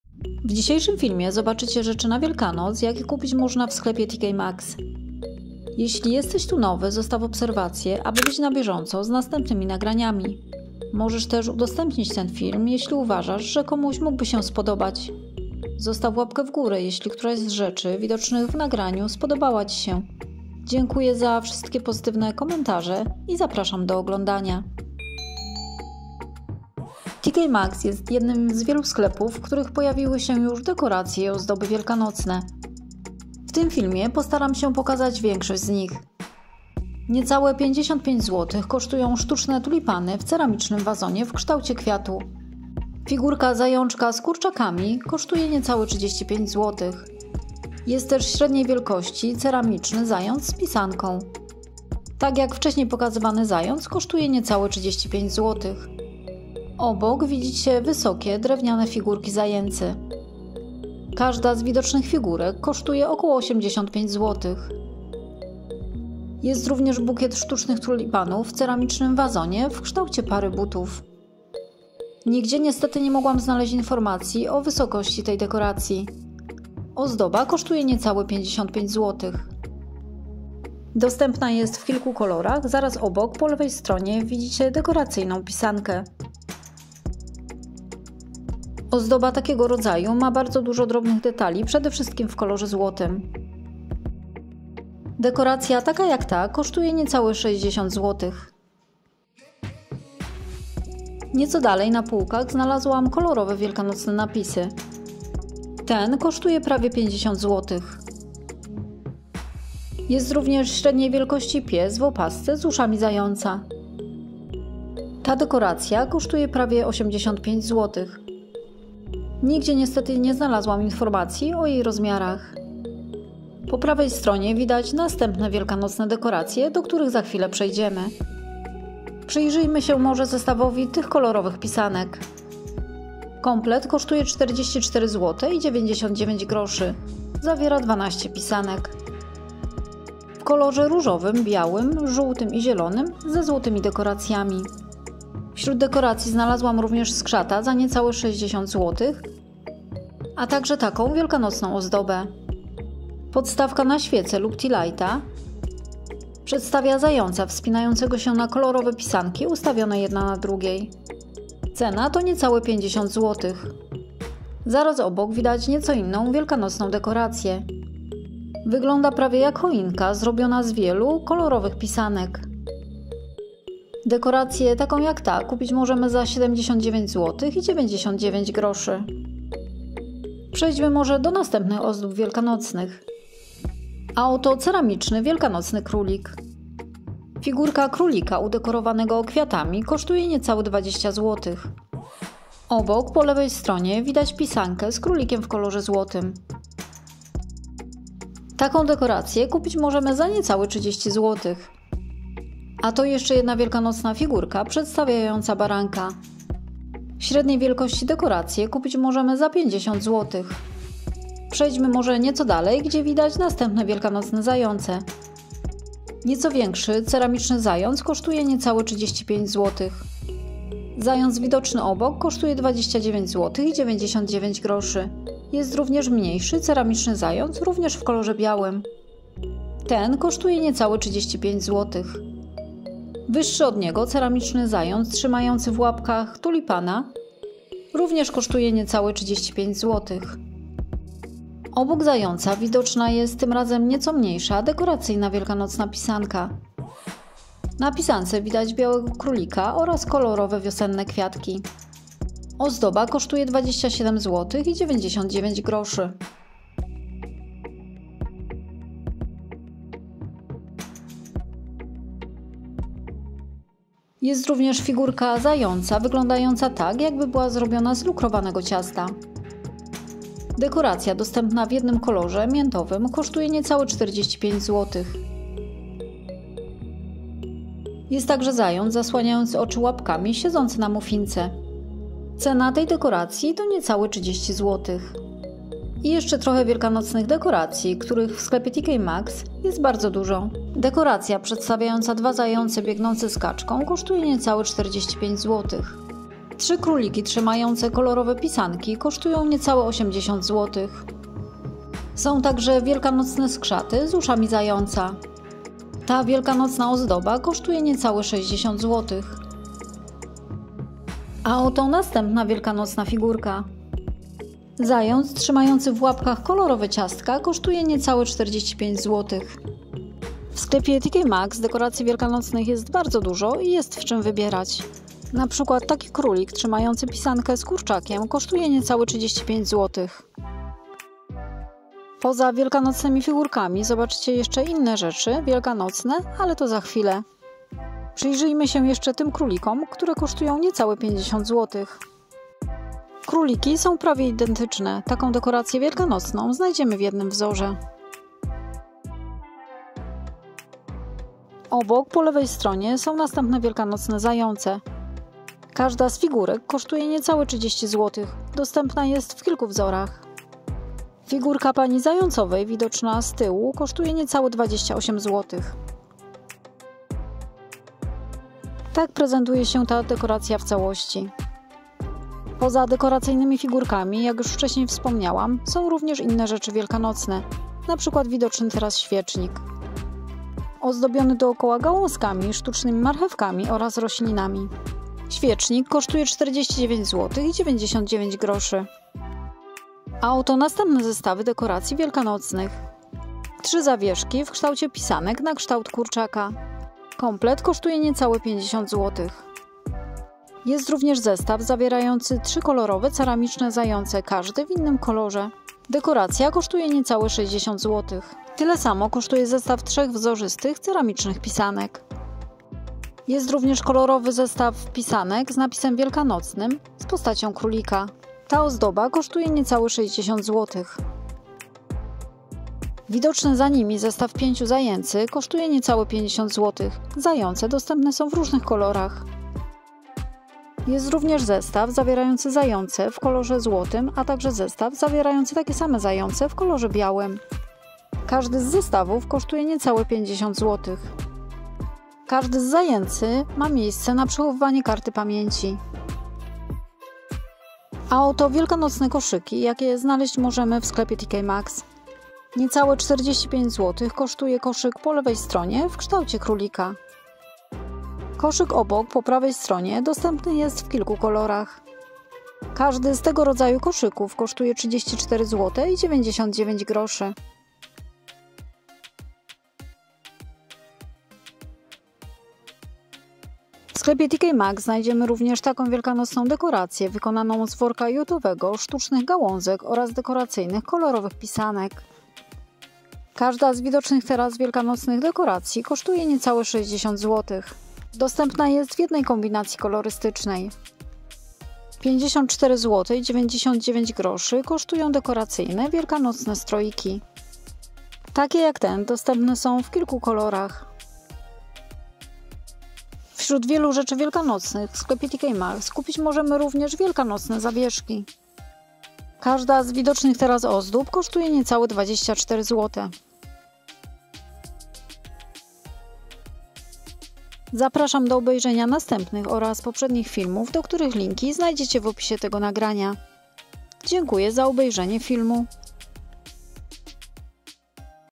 DEKORACJE i OZDOBY WIELKANOCNE w Tk Maxx 🌿🐰NOWOŚCI na WIELKANOC Przegląd sklepów i półek
Dekoracje i ozdoby  na Wielkanoc pisanki zające stroiki kurczaki koszyki i kosze zestawy akcesoria do domu w sklepie Tk maxx